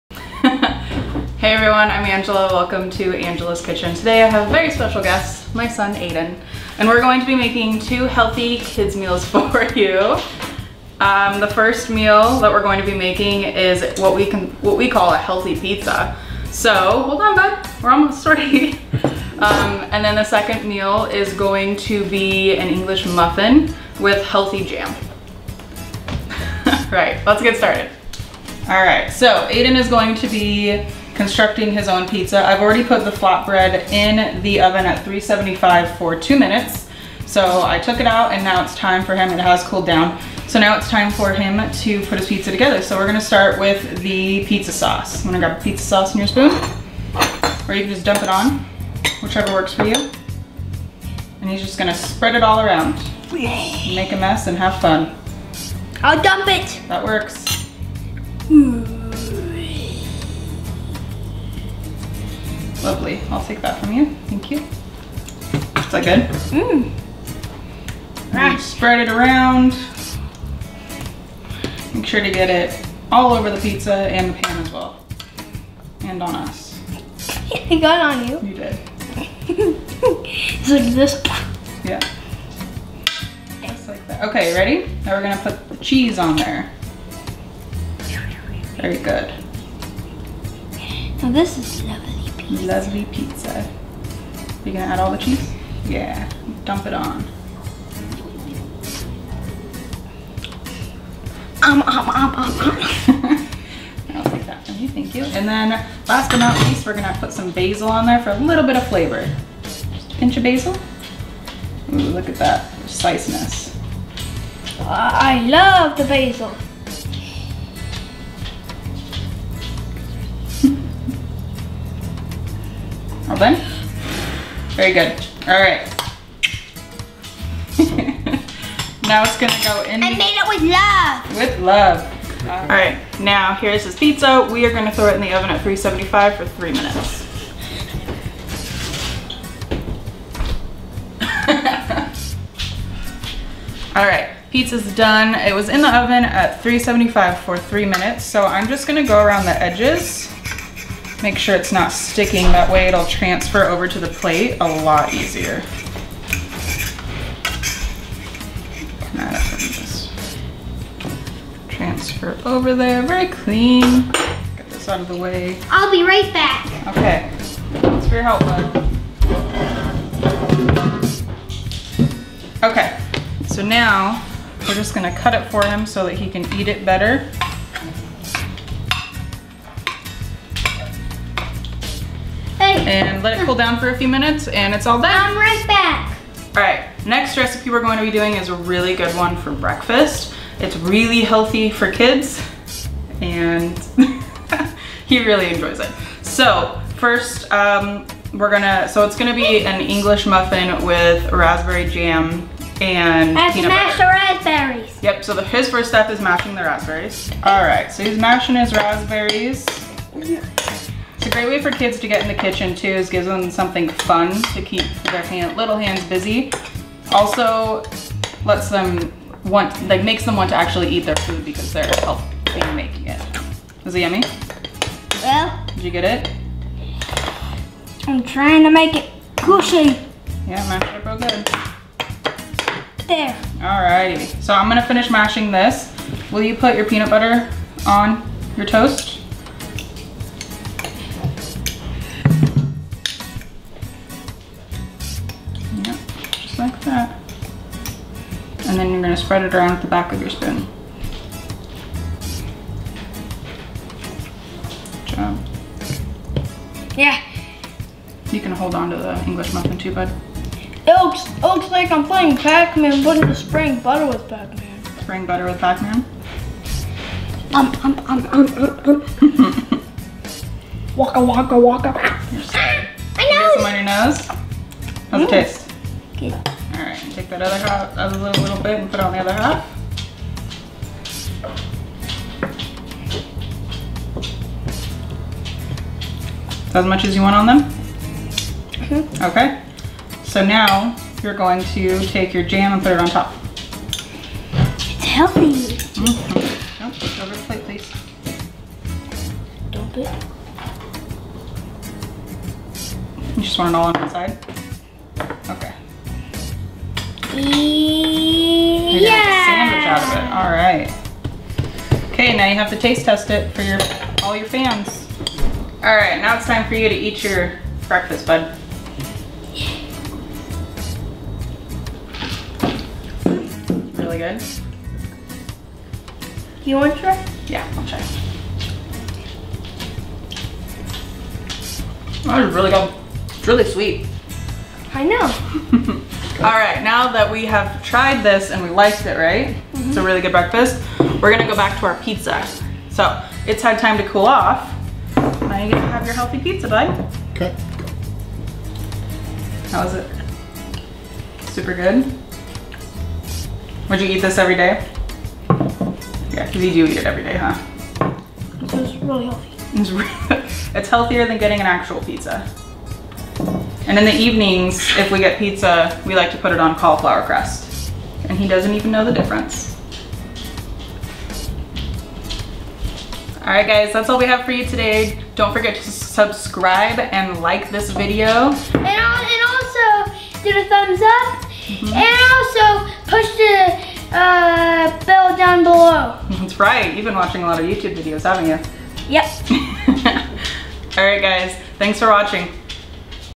Goofy Kid Making Flatbread Pizza and English Muffin Pizza
Angela is showing us how to make Kid Friendly Pizza and English Muffins, perfect for the busy mom who wants a healthy choice to feed their kids and doesn't take a lot of time
Pizza -
Cook the flat bread at 350 until it is at the consistancy you like
Add Pizza Sauce, Cheese and Basel and whatever else you would like to the pizza. Place back into the over until done
Remove, cut, cool and eat
English Muffin
Take fruit and mash it up with a fork
Place peanut butter onto the muffin
add mashed fruit to muffin
Eat